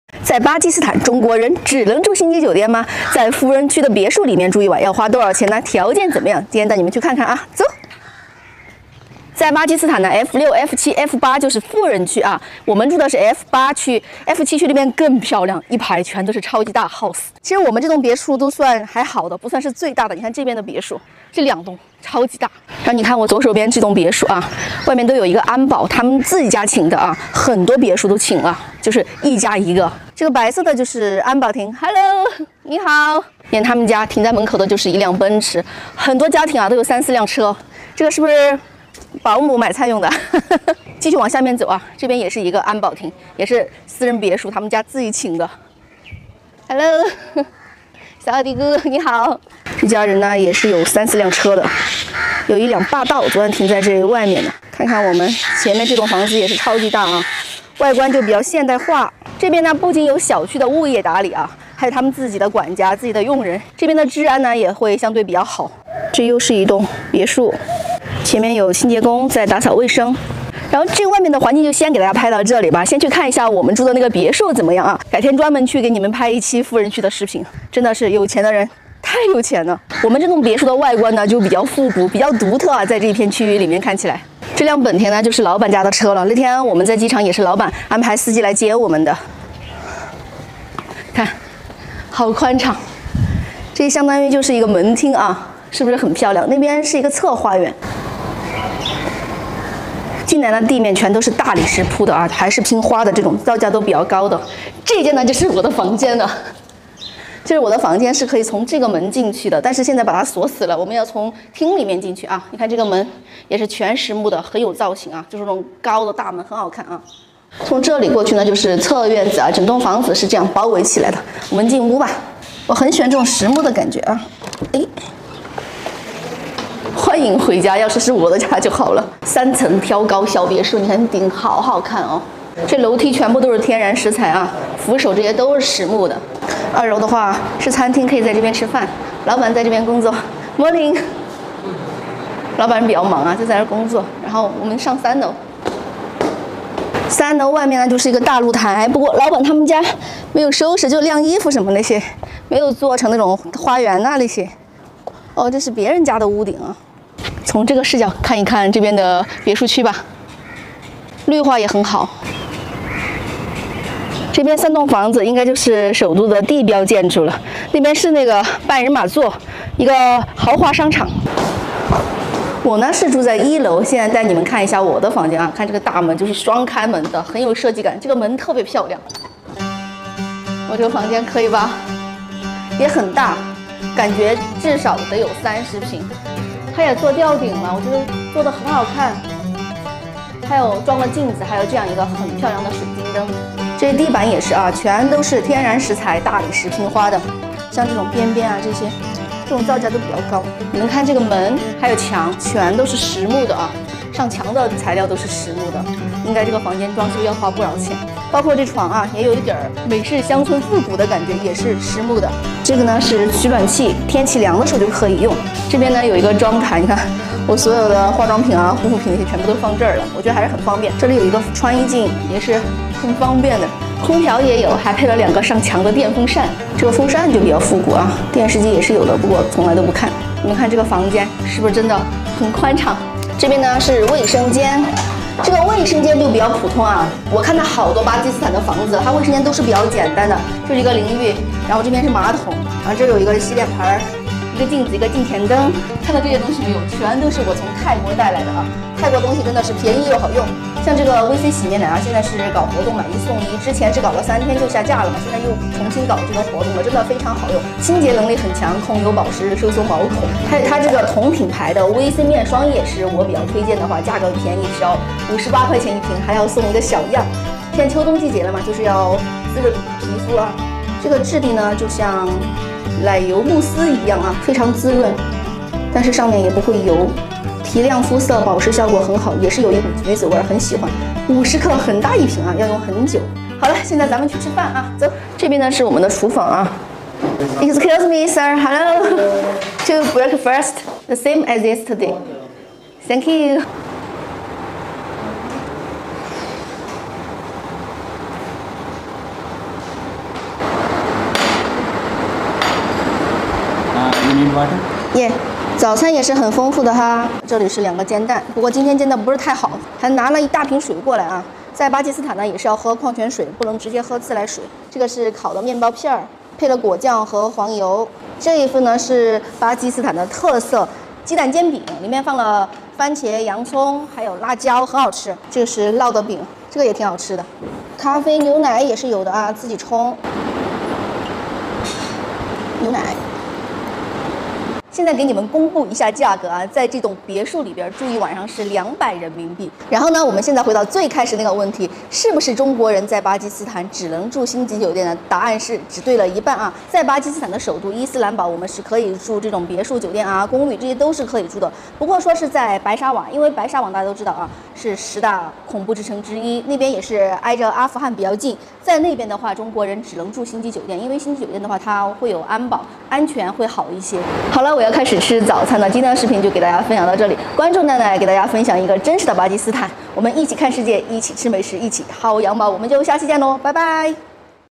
實拍巴基斯坦富人區！在月均收入700元的巴基斯坦，住一晚別墅多少錢？｜出發吧奈奈
巴基斯坦人均月收入700元，首都富人區是什麼樣子？別墅住一晚多少錢？